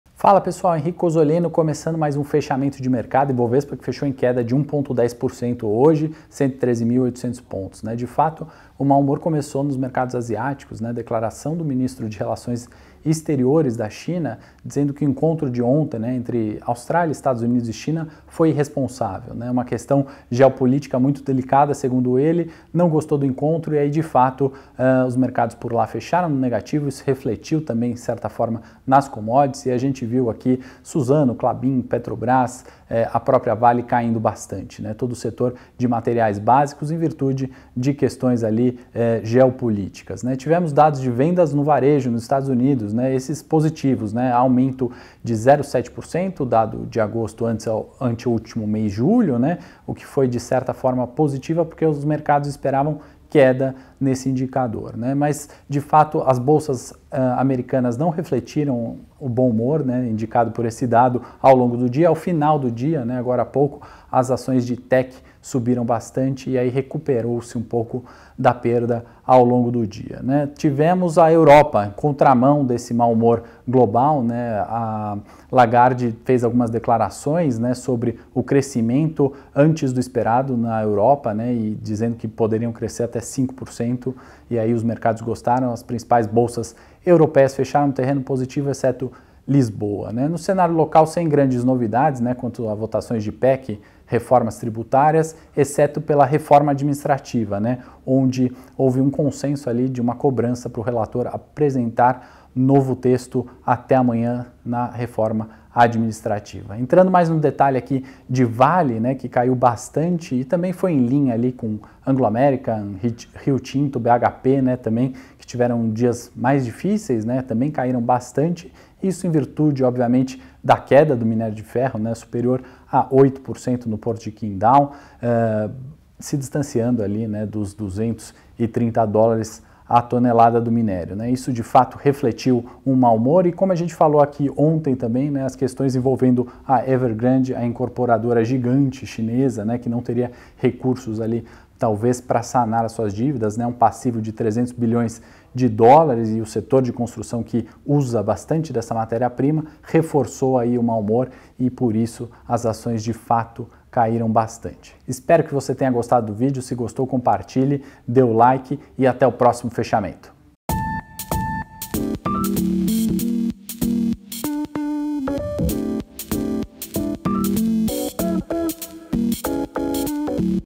Ibovespa tem mais uma sessão de queda, puxado pela Vale e Petro
Mercado Local

 Ibovespa   113.794 pontos   -1,10%

O Ibovespa teve mais uma sessão de queda, puxado pelas gigantes Vale e Petro, que possuem grande peso no índice.

Mercado Externo

 Nasdaq 0,13%

 S&P500 -0,16%

 Dow Jones -0,18%

Câmbio

  Dólar R$ 5,26   +0,54%

Commodities

 Petróleo Brent (Barril)  US$ 75,65     0,25%

 Minério de ferro (Ton): US$ 107,21    -8,09%

Dia de mais uma grande queda no minério de ferro, com a China apresentando dados mais fracos de produção de aço.

Vencedora da enquete do Telegram

A Vale fechou o dia com queda maior de 4%, seguindo a derrocada do preço do minério de ferro na China.

Maiores Altas

CIEL3

R$ 2,52

+5,4%

HGTX3

R$ 38,52

+4,9%

ASAI3

R$ 19,16

+3,1%

BEEF3

R$ 9,84

+2,5%

BBAS3

R$ 29,47

+1,6%

Após a aprovação da combinação de negócios com a Soma, a Hering encerrou mais um dia de alta, dessa vez de quase 5%.

Maiores Baixas

CSNA3

R$ 31,28

-6,2%

SUZB3

R$ 54,78

-5,8%

USIM5

R$ 14,70

-5,4%

CASH3

R$ 7,37

-4,7%

KLBN11

R$ 23,97

-4,6%

A maior queda da bolsa ficou por conta da CSN, após o minério de ferro desabar mais de 8%.